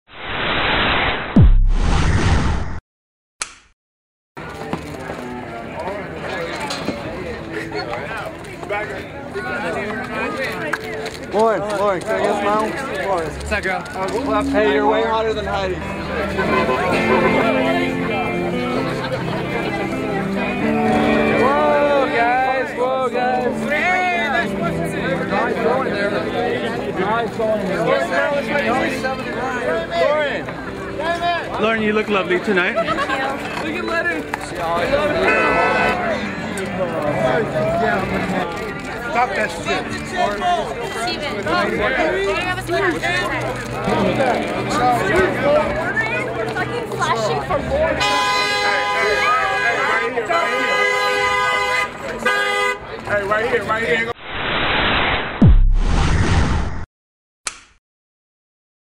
Lauren Conrad leaving Club Opera in Hollywood
Lauren Conrad leaving Club Opera in Hollywood.Sept,21,2007.